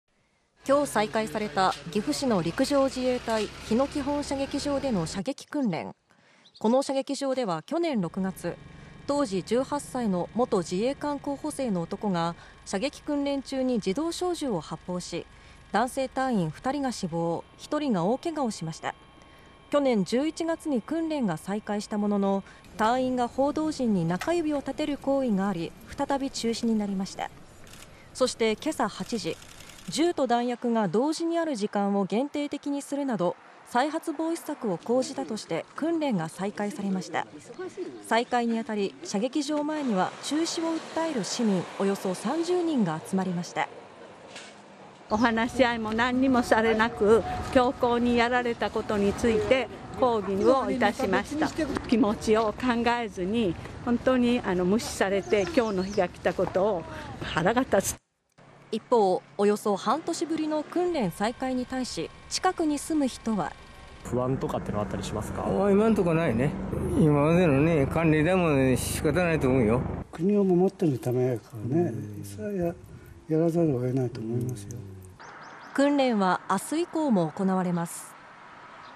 「話し合いもなく強行に再開された」 岐阜市の日野基本射撃場で訓練再開 去年6月に隊員3人が死傷
隊員3人が死傷する事件が起きた、陸上自衛隊の射撃場で訓練が再開されました。22日、再開された岐阜市の陸上自衛隊日野（ひの）基本射撃場での射撃訓練。
この射撃場では去年6月、当時18歳の元自衛官候補生の男が、射撃訓練中に自動小銃を発砲し、男性隊員2人が死亡、1人が大けがをしました。

去年11月に訓練が再開したものの、隊員が報道陣に中指を立てる行為があり再び中止になりました。そして22日8時、銃と弾薬が同時にある時間を限定的にするなど再発防止策を講じたとして、訓練が再開されました。

再開にあたり射撃場前には、中止を訴える市民約30人が集まりました。（中止を訴える市民）
「話し合いも何もなく（訓練を）強行に再開されたことに抗議をした。気持ちを考えずに無視されてきょうの日が来たことに腹が立つ」一方、約半年ぶりの訓練再開に対し、近くに住む人は…

Q.不安はある？
（近隣住民）
「いまのところない。いままでの慣例なので仕方ない」
「国を守るためだから（訓練は）やらざるを得ないと思う」訓練は、23日以降も行われます。